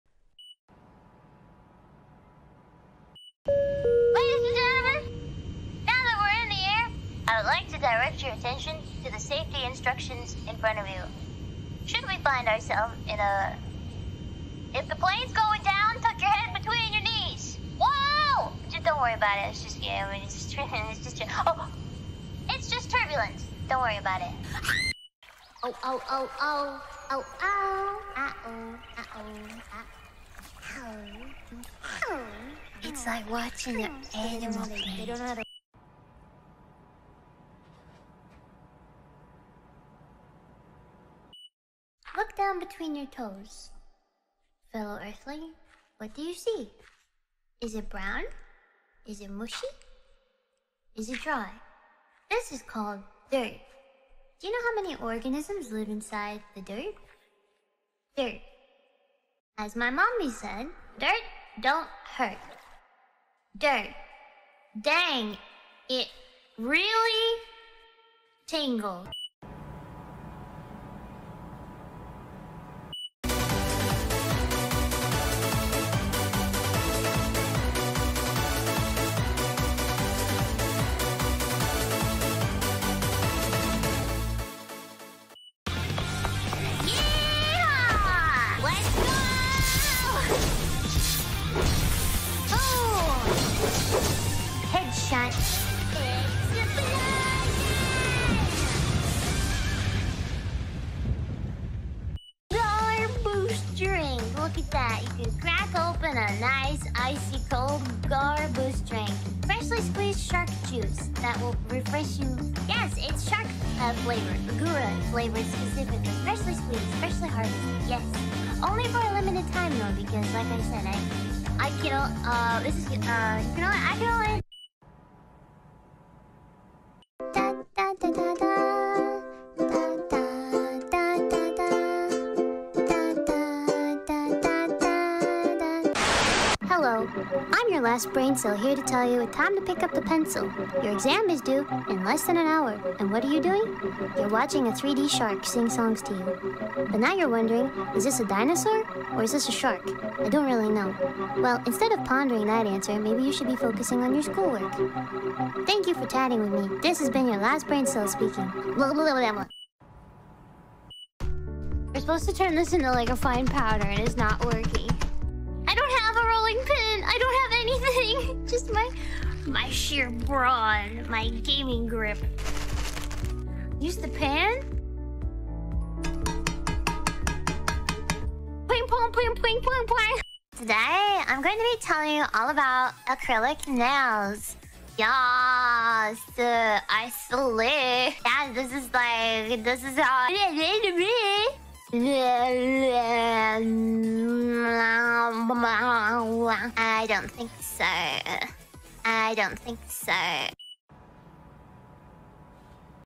Gawr Gura TV
Collection of all the tv channels Gura flipped through during her 3d debut!